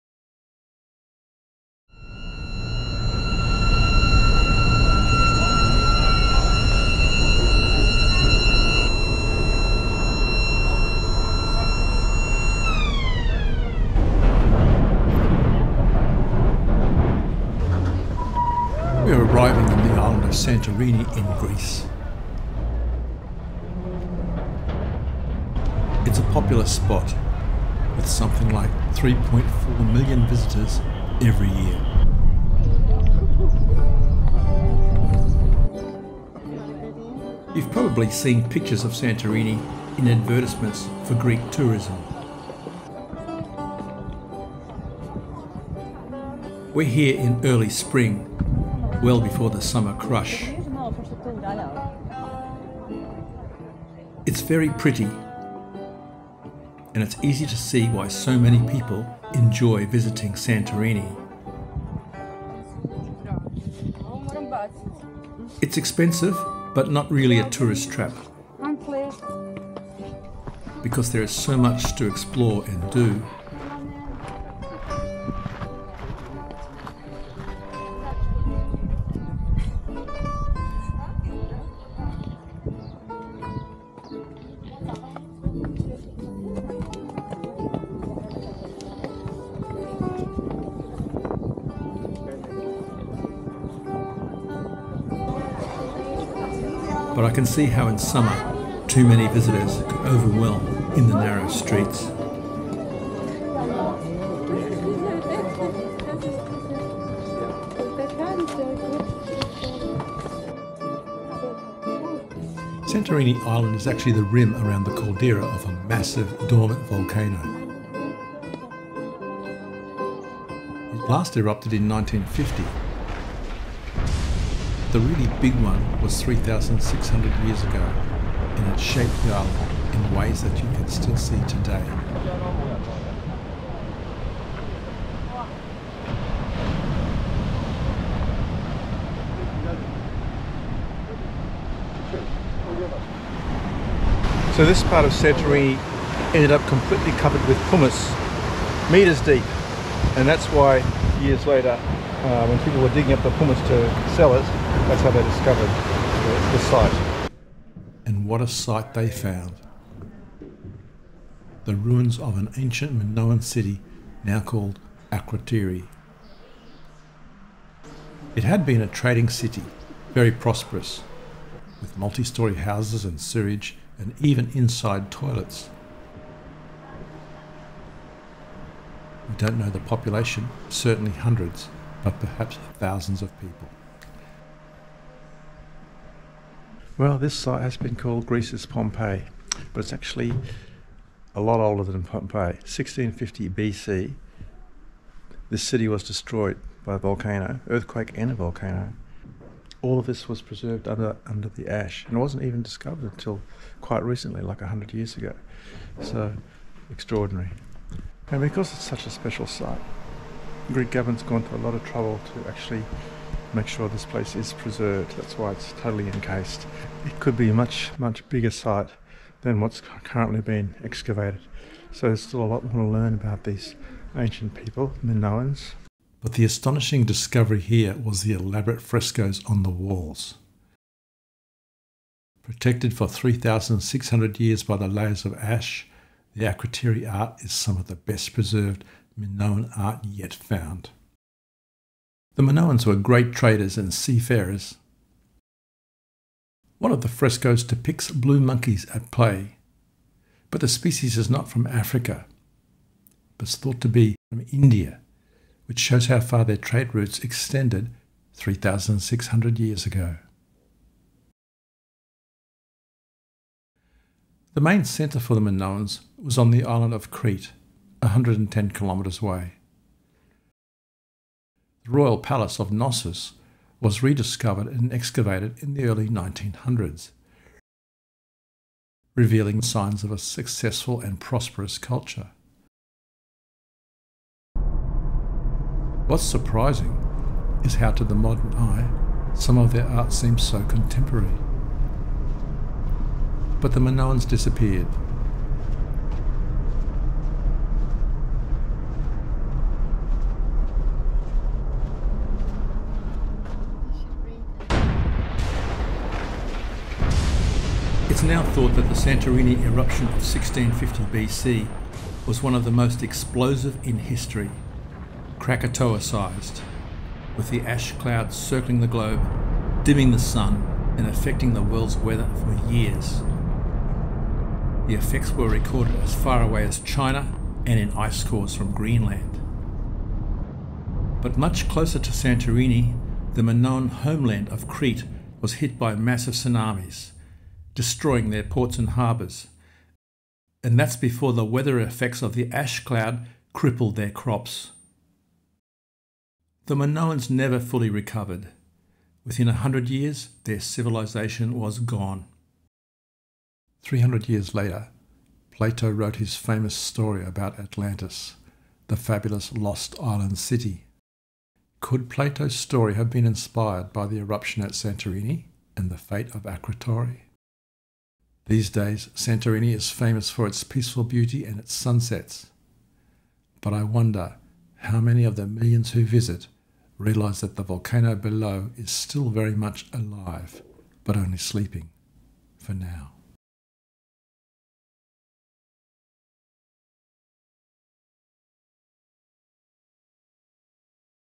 Santorini, Akrotiri and the mystery of Atlantis
Santorini is much more than just a pretty place for tourists. The discovery of an ancient city hidden beneath may explain two of the world’s great unsolved mysteries. What happened to the Minoans? And where was the legendary city of Atlantis?

00:00 Arriving on Santorini by ferry
00:34 Visiting in Spring
01:00 Is it a tourist trap?
01:25 Maybe in the Summer
01:53 Santorini on the caldera of a volcano
02:20 The discovery of ancient Akrotiri
03:45 The amazing preserved art of the Minoans
04:24 The Minoan homeland - Crete & Knossis
05:05 The Krakatoa-sized explosive eruption of Santorini in 1650 BCE
05:50 Plato and the legend of Atlantis